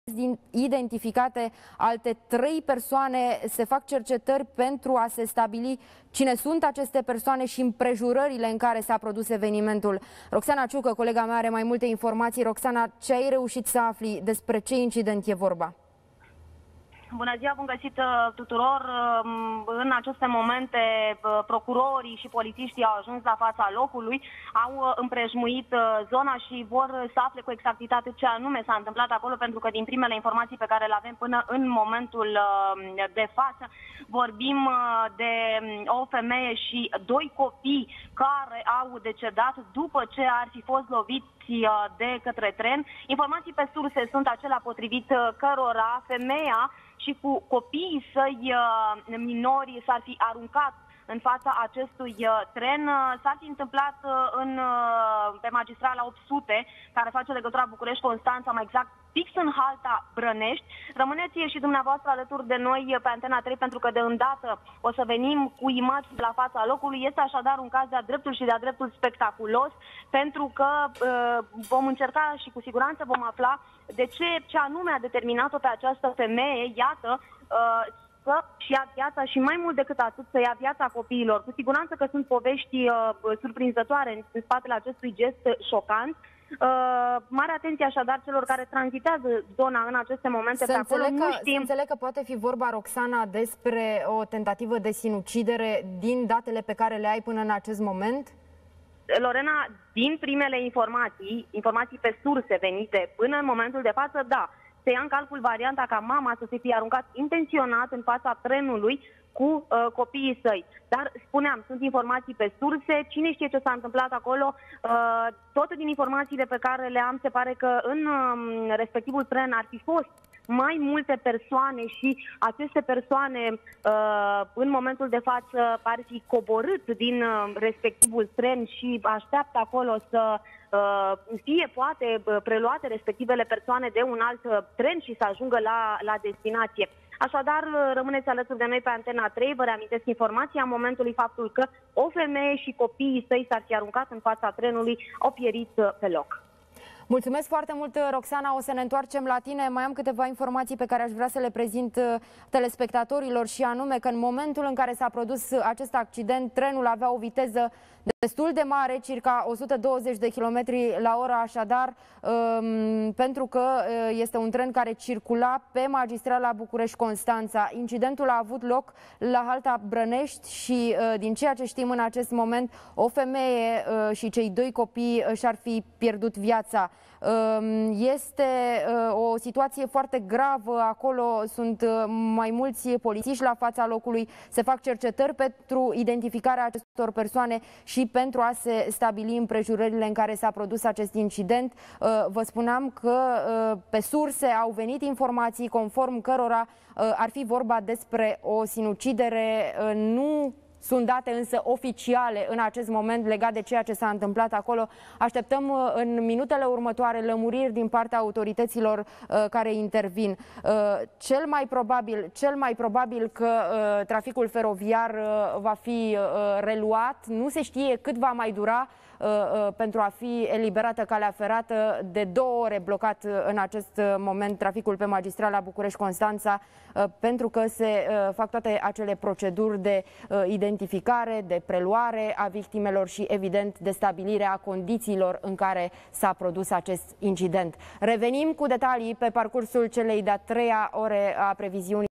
Tragedie pe calea ferată! Circulația blocată de două ore pe ruta București-Constanța
Tragedie pe calea ferată! Circulația blocată de două ore pe ruta București-Constanța, Previziuni, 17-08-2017